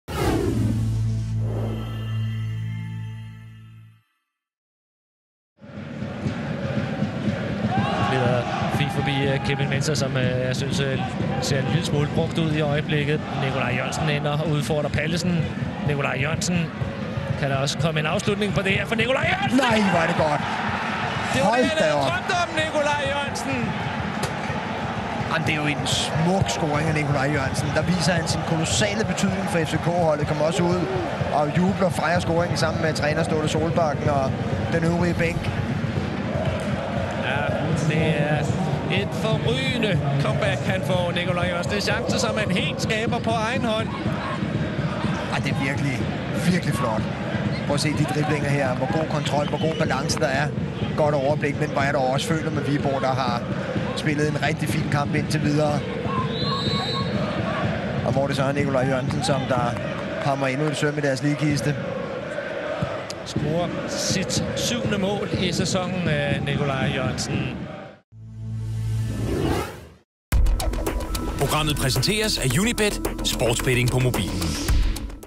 Se Nicolai Jørgensens solomål i sit comeback
Nicolai Jørgensen var ikke længe om at vise sit værd, da han torsdag aften gjorde comeback i kampen mod Viborg. Nyd hans flotte solomål her: